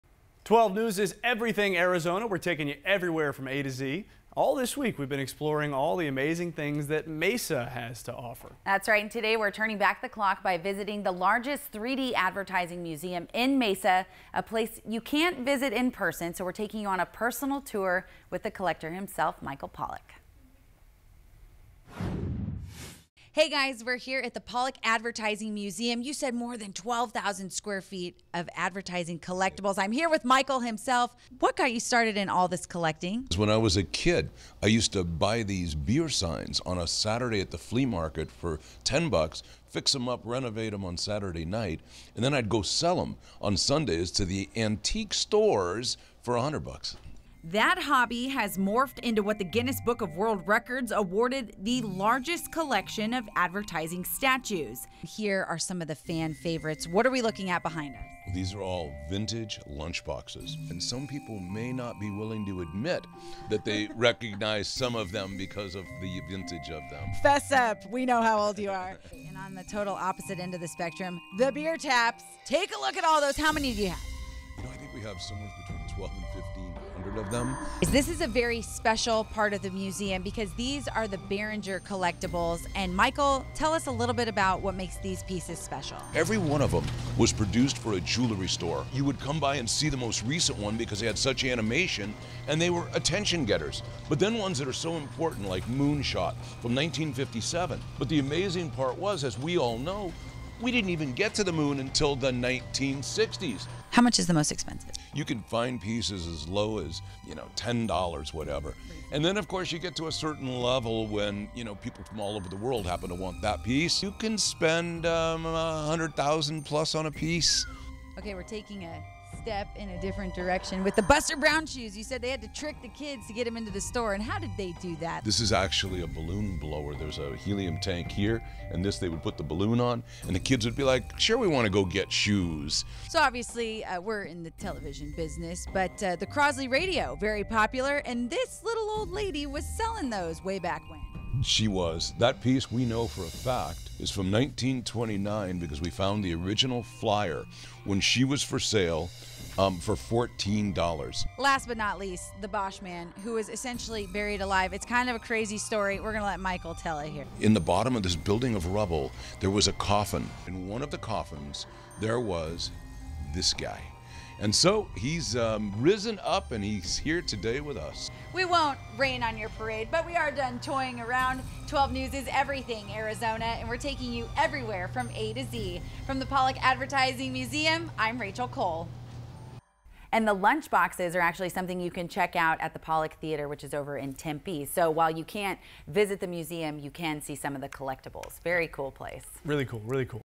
Everywhere A to Z: The Pollack Advertising Museum is full of wonder
Rachel Cole chats with owner Michael Pollack to learn more about the history of the Pollack Advertising Museum in Mesa.